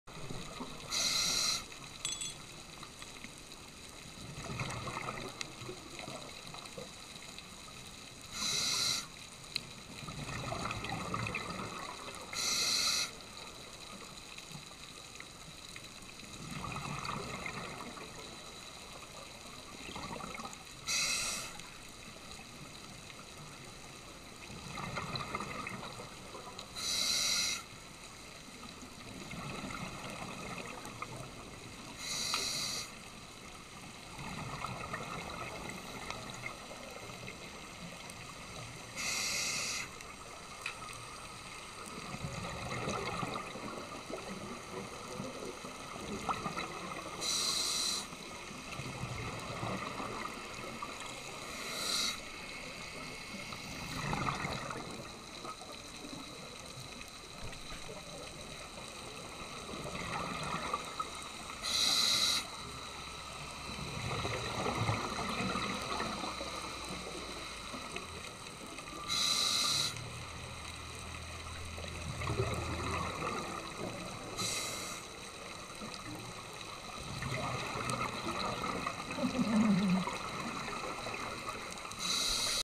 Rays, Rays and More Rays
Virador was beautiful yesterday...rays, rays, rays...we saw butterfly rays, southern stingrays, eagle rays, bulls eye rays, shovel rays, AND manta rays...Water was warm :) The new filter is awesome!